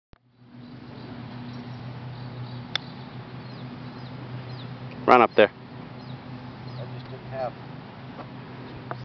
U.S. Open Senior Qualifier - Greensboro Country Club 05
Sunday, June 26, 2011 - Every once in a while in the game of golf, you'll hit a shot into a place that makes even a Tour Player think twice.  In this situation, the ball came to rest just above the bunker short of the green.  As you can see, the heels of the feet are hanging over the edge.  You must maintain great balance with your body to allow your arms and hands to swing the club back and through.  This was a good shot and the ball came to rest about 8 feet from the hole.